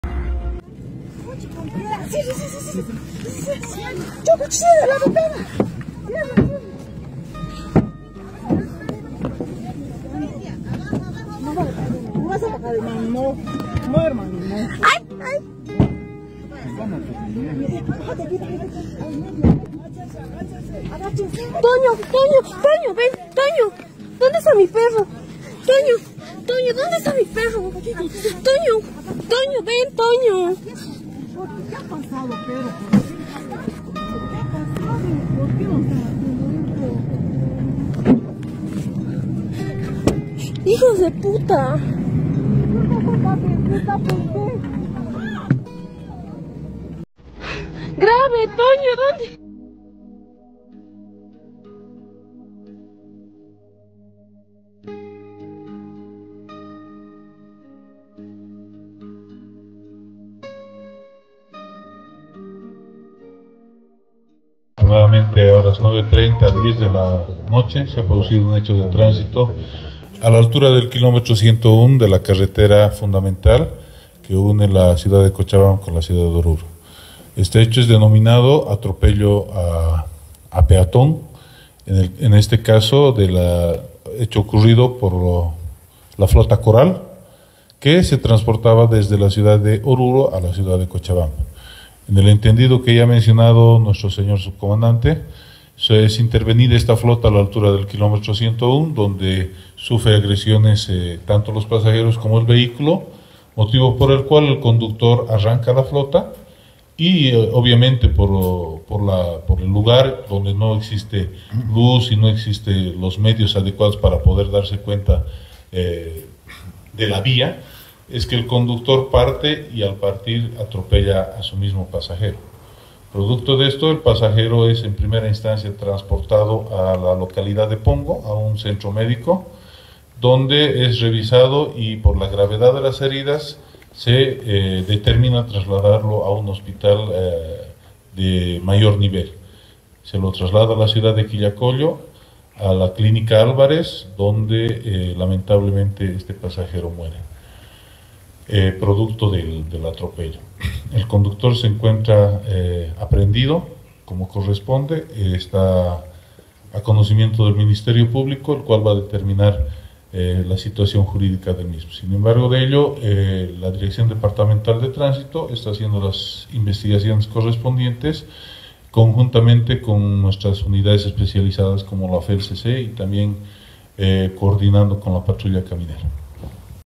Un pasajero murió en el Asalto a la Flota Coral en la carretera Oruro - Cochabamba esta mañana
Uno de los pasajeros del bus Coral perdió la vida al ser atropellado, ocurrió cuando el chófer intentaba alejarse de los asaltantes.